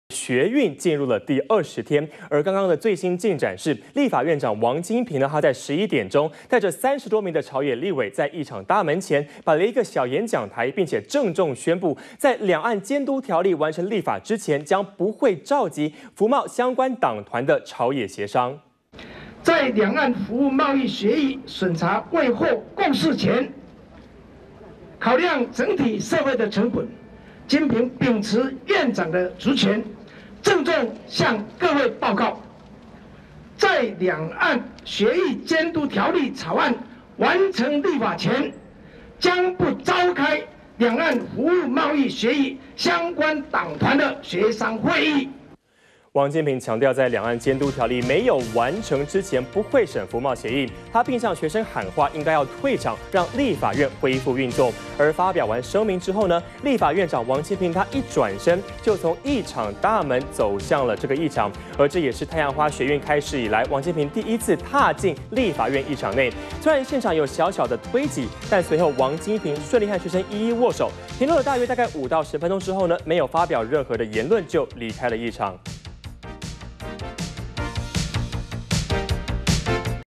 【2014.04.06】王金平:監督條例立法前 不審服貿 -udn tv
學運進入第20天，剛剛的最新進展是，立法院長王金平在11點鐘，帶著30多名朝野立委，在議場大門前，擺了小演講台，並且鄭重宣布，在兩岸監督條例完成立法前，將不會召集，服貿相關黨團的朝野協商。